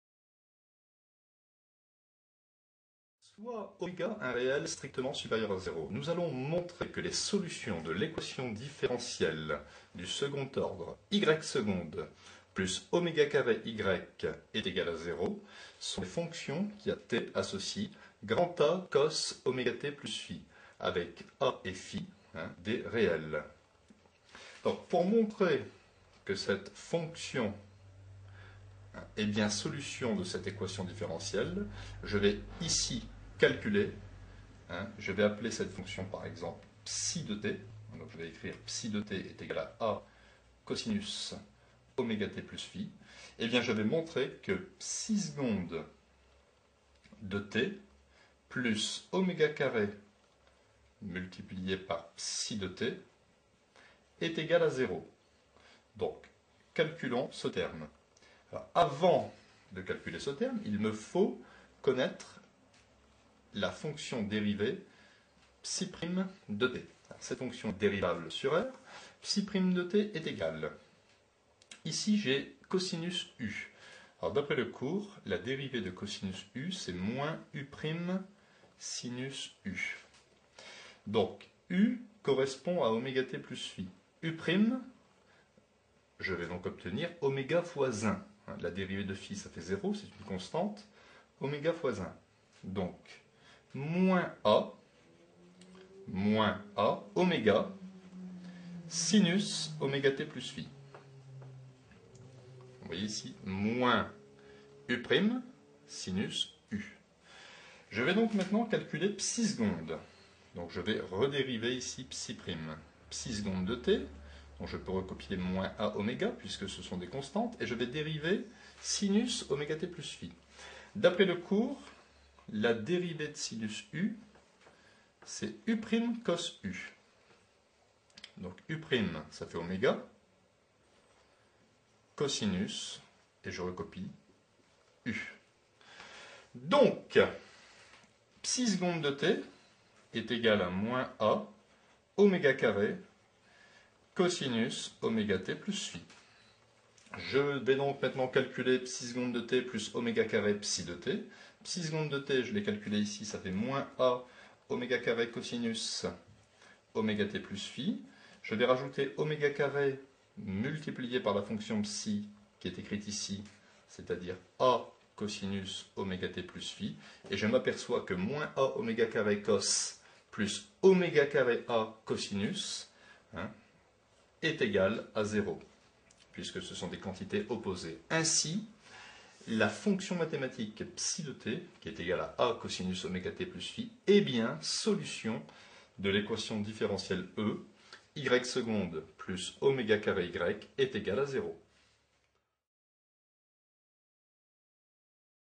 Fonction trigonométrique et équation différentielle.Maths TS
Nous devons montrer qu'une fonction trigonométrique donnée est solution d'une équation différentielle du second ordre.